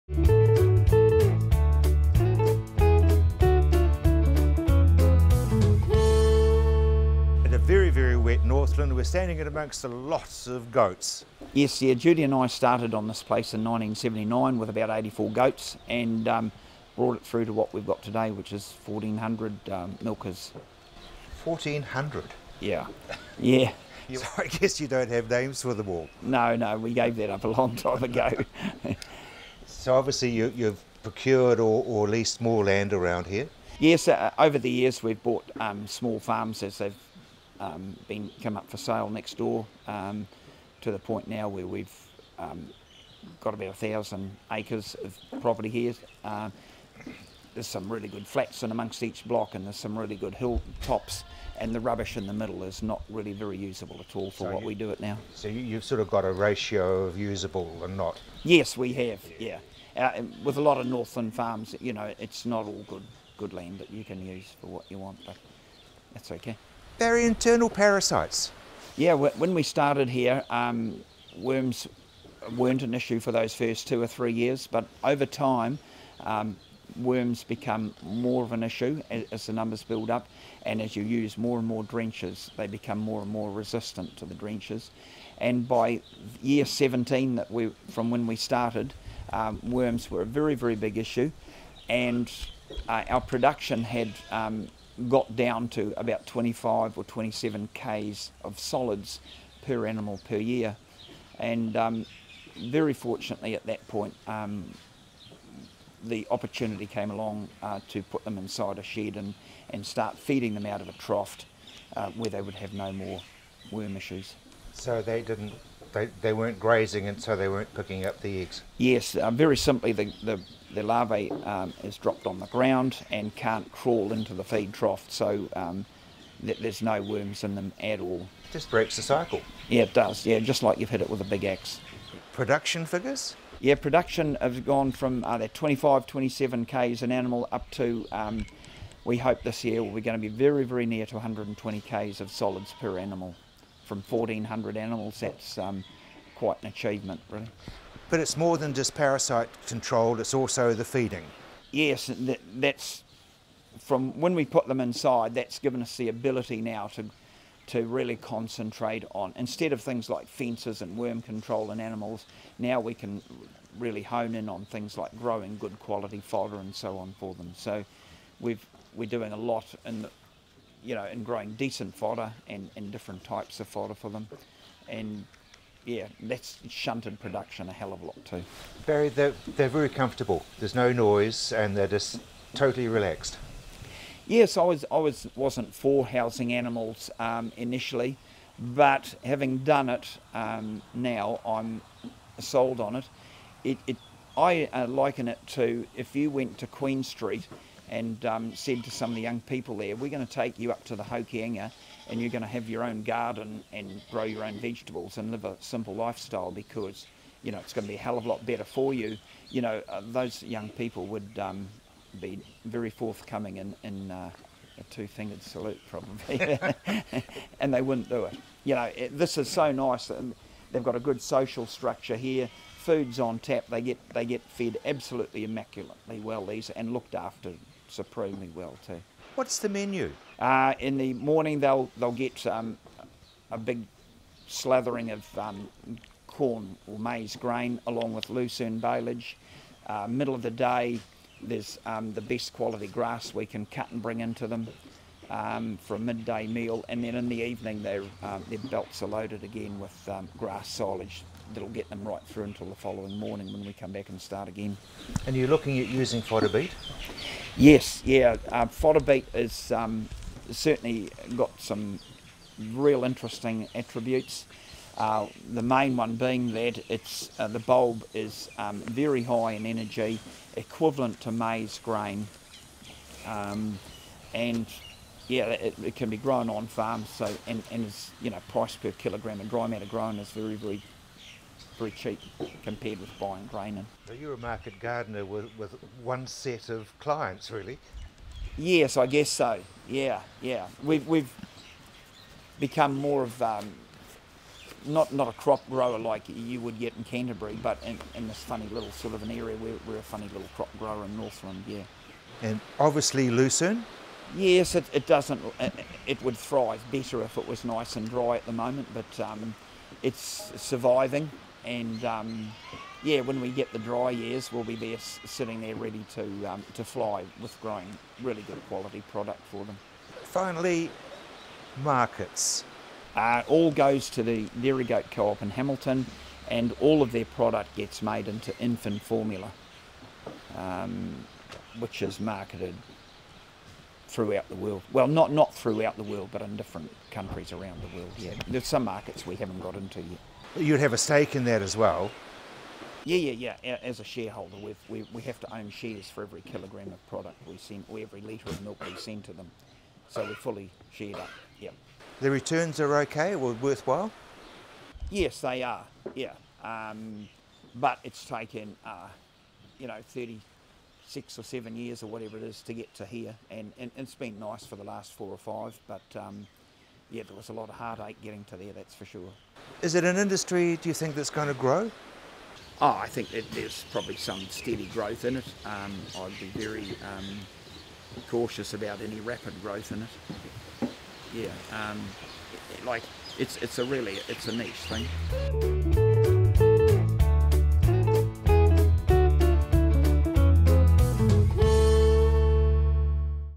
Barry Foote: Northland Goat Farmer
Livestock - Growing different grasses can ensure success.

Farming matters on any screen, anywhere, anytime.